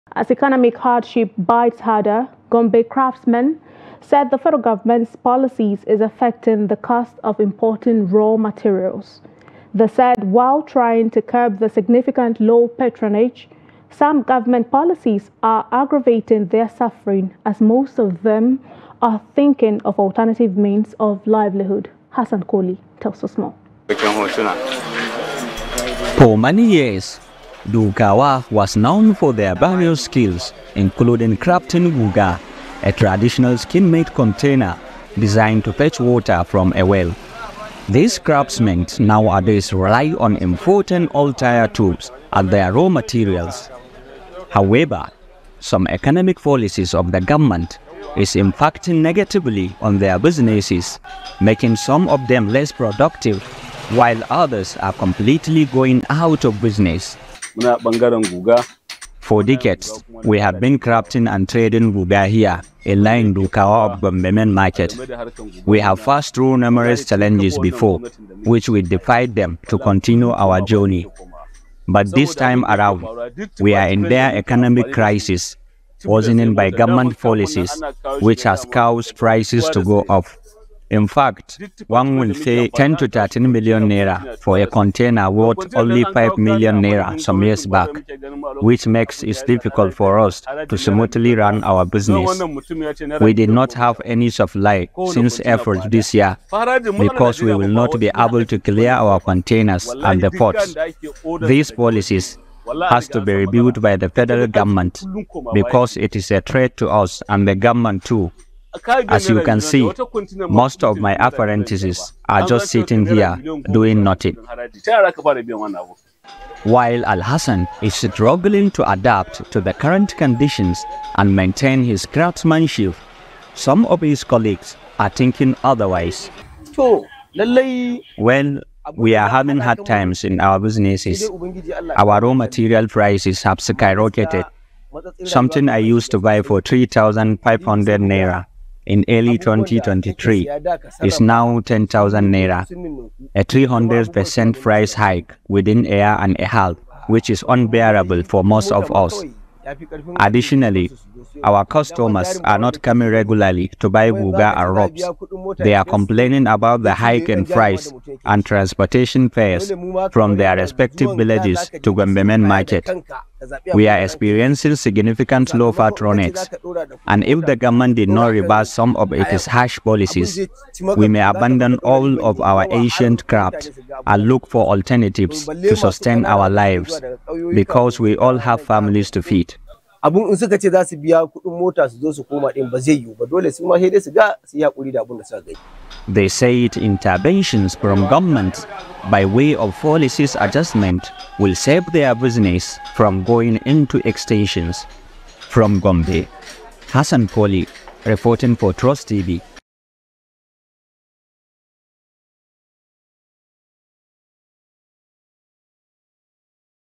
Gombe Craftsmen Lament Impact Of Economic Policies On Business
As economic hardship bites harder, Gombe craftsmen said the federal government policies is affecting the cost of importing raw materials.

They said while trying to curb the significant low patronage, some government policies are aggravating their suffering as most of them are thinking of alternative means of livelihood.

For many years, Dukawa was known for their various skills, including crafting 'Guga', a traditional skin-made container designed to fetch water from a well.

These craftsmen nowadays rely on imported old tire tubes as their raw materials.

However, some economic policies of government is Impacting negatively on their businesses, making some of them less productive while others are completely going out of business.